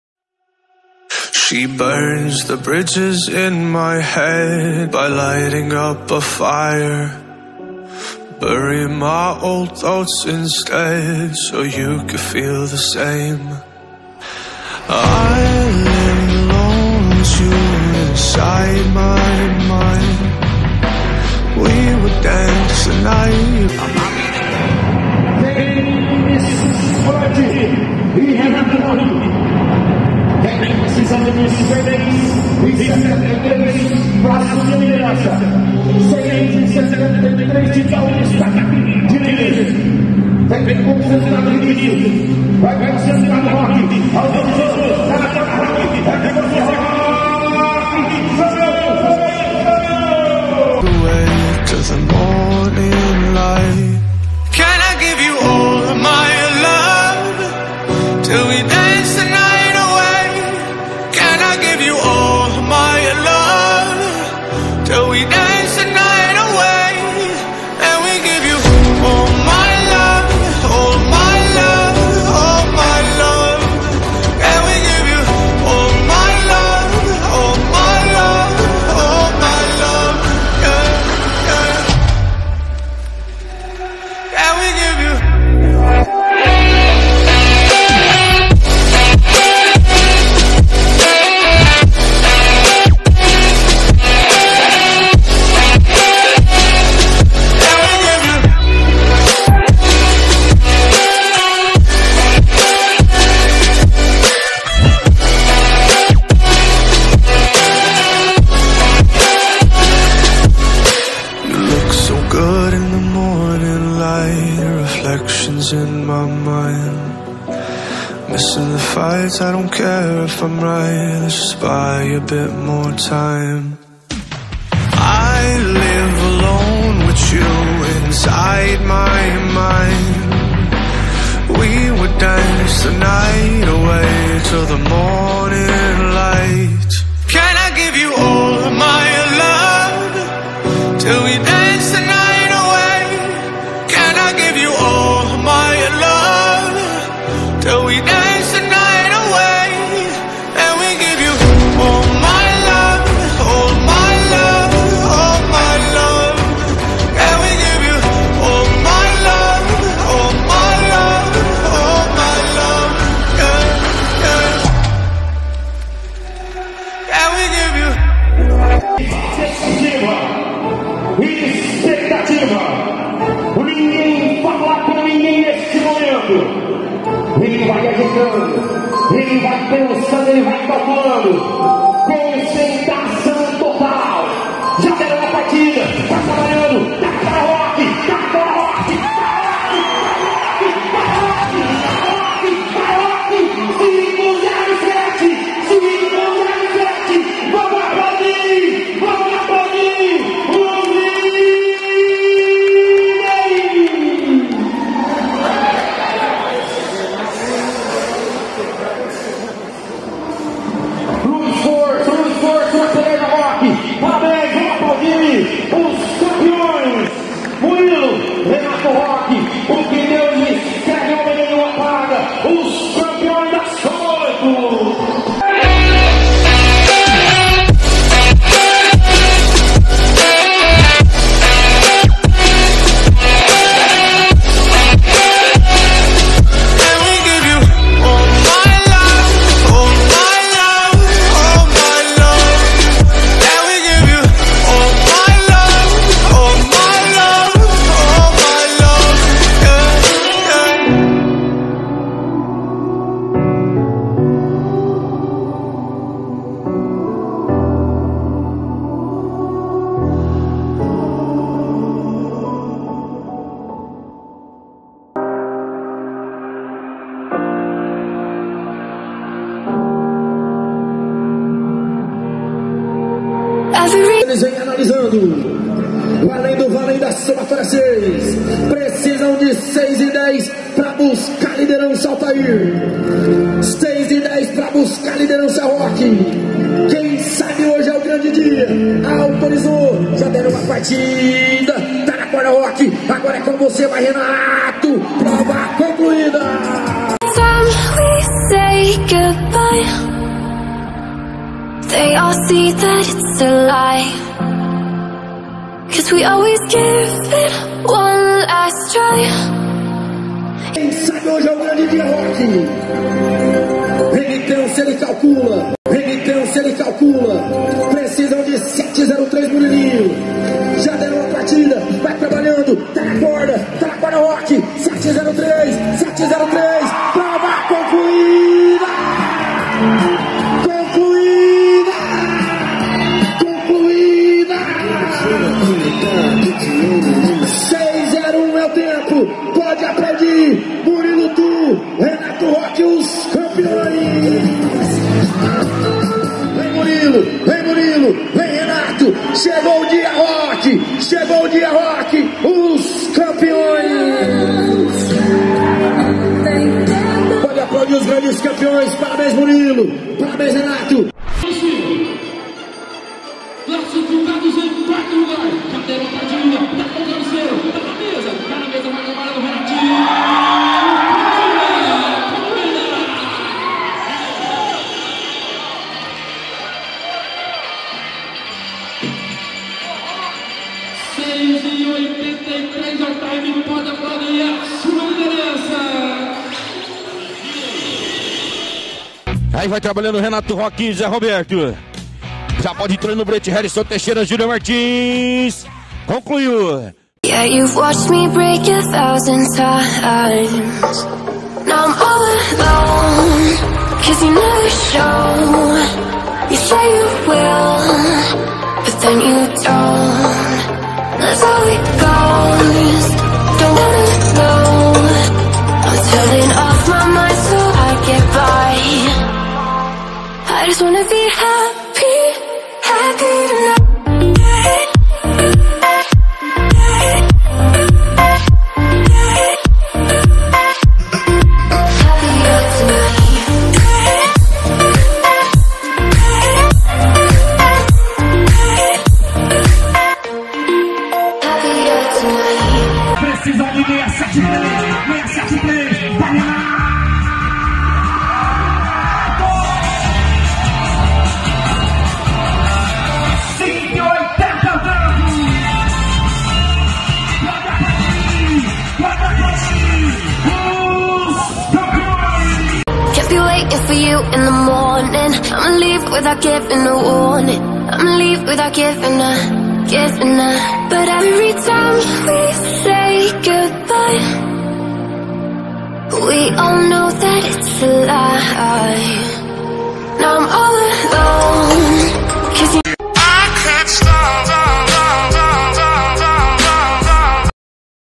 RENATO CANDIDO- NO MOCOTÓ DOS BOVINOS!!!
Nesse vídeo crio um ''vídeo clipe'' mostrando o trabalho de Renato Candido um grande laçador de pe de boi brasileiro,tendo se hd estimado de 4 boi,na qual mostra muitas habilidades!